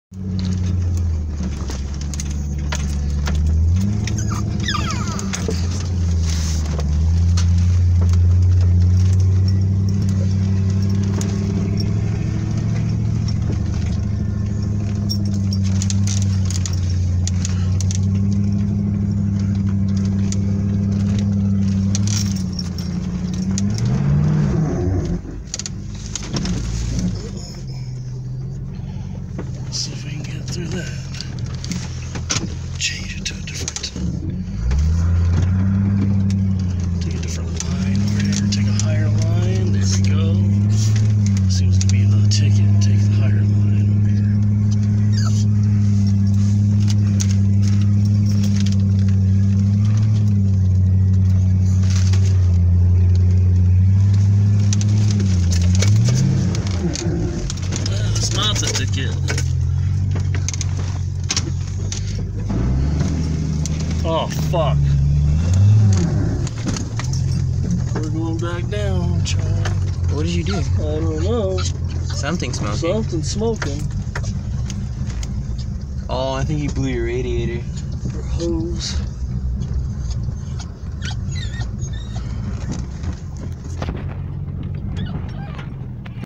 Blew a radiator on an offshoot
Blew a radiator hose on the WJ. Luckily it was repairable and we were able to hit the rest of the offshoots and trails